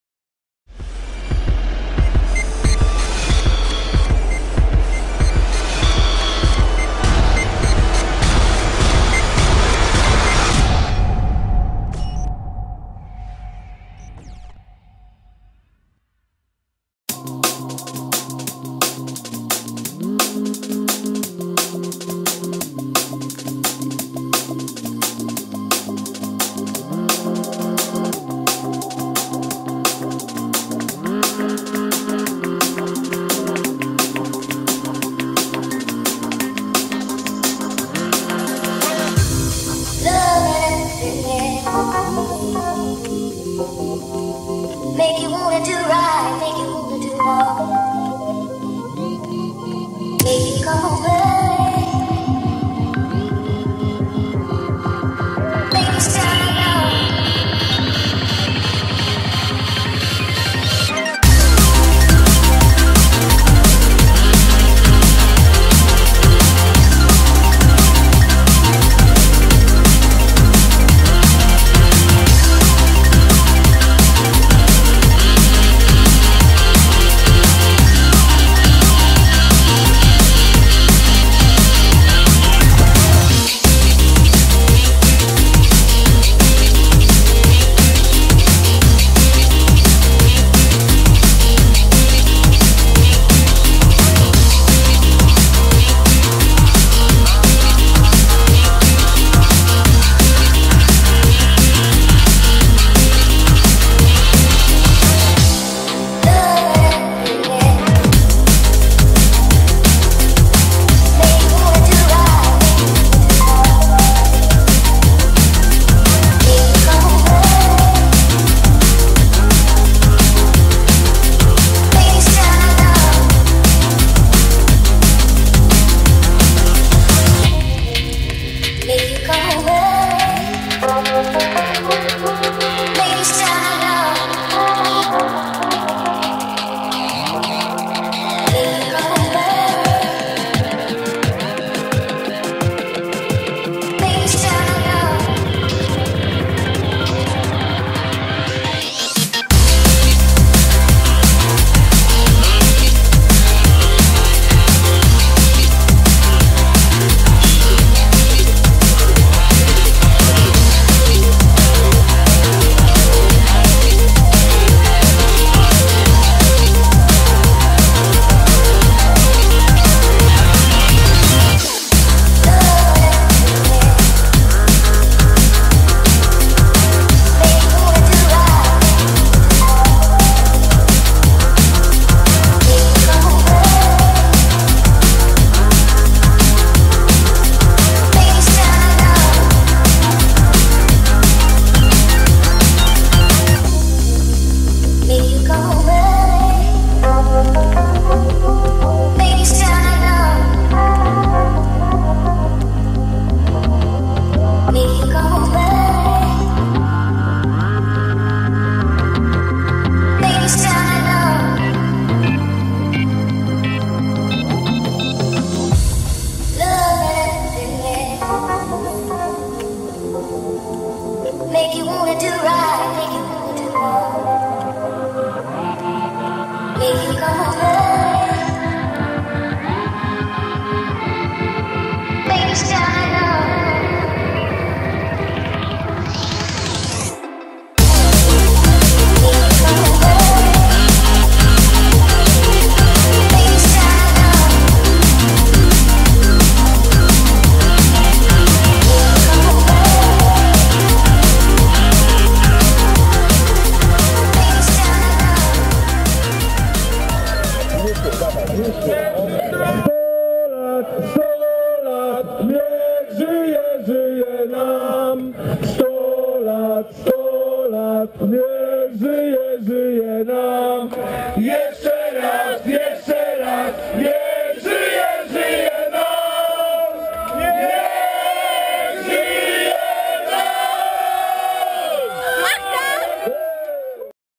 PERFORM FESTIVAL 2012 - 6 urodziny Agencji Perform (CHONABIBE & CREW)
W dniach 03-05 sierpnia 2012 odbyła się pierwsza edycja PERFORM FESTIVAL, czyli urodzinowa impreza Agencji Perform. W ramach festiwalu na scenie koncertowali CHONABIBE oraz CREW. Kto nie dojechał zdecydowanie ma czego żałować !!!